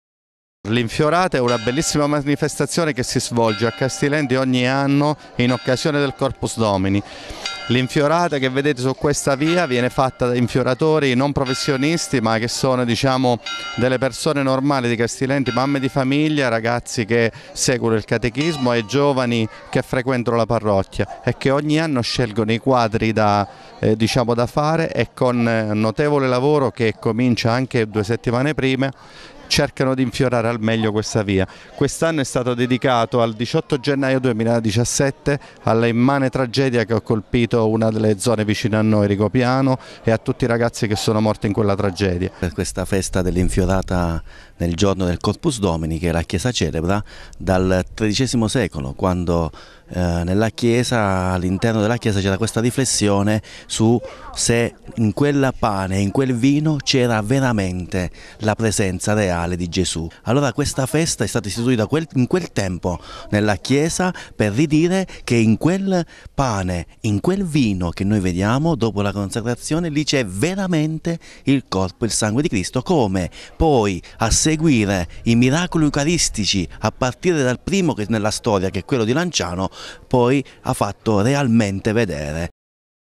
L’infiorata di Castilenti
Anche quest’anno in occasione del Corpus Domini a Castilenti è stata organizzata la cosiddetta “infiorata”. Vediamo di cosa si tratta